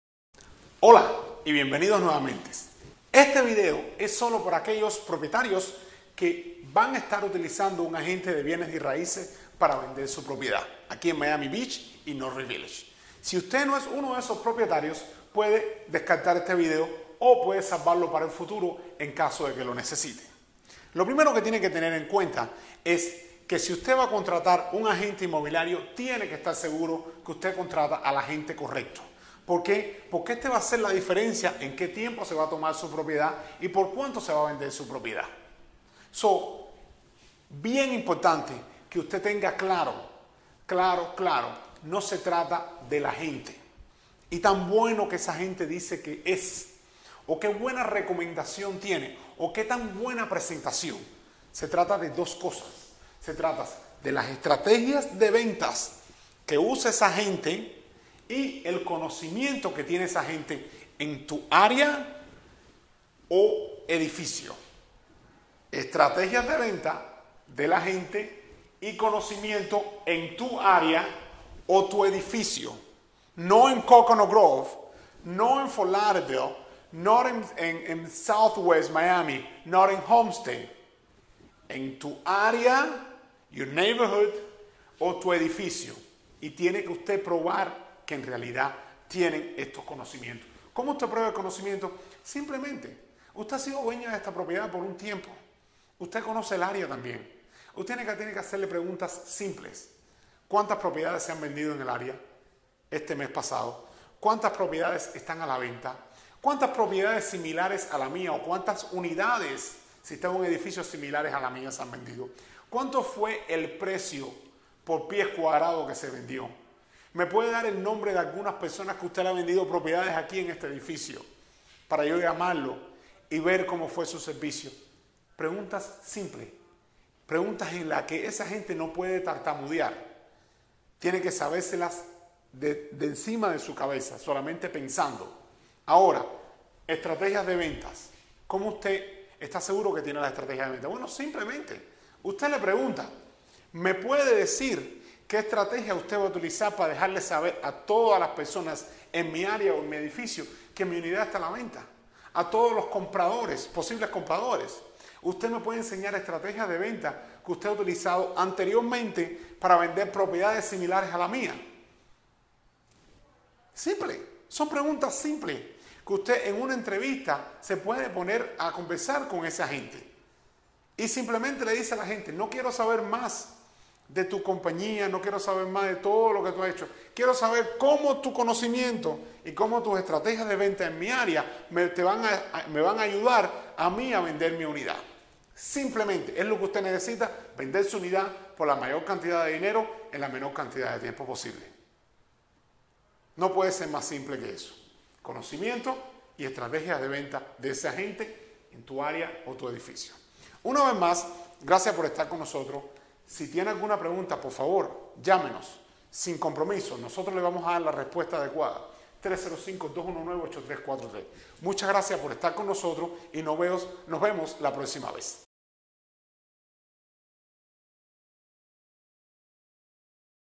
4- ¿Cómo saber si estás contratando el agente correcto?
El agente inmobiliario que usted contrate para vender su propiedad en Miami Beach o North Bay Village determinará cuánto tiempo permanecerá en el mercado y por cuánto está se vaya a vender.

Haga clic en el vídeo de arriba, donde usted aprenderá:

¿Qué buscar cuando está entrevistando el agente adecuado?

¿Cómo probar sus conocimientos en el area o edificio?

¿Cómo poner a prueba sus habilidades de venta?

Visite para obtener Los 12 Pasos que usted debe seguir a la hora de vender su propiedad.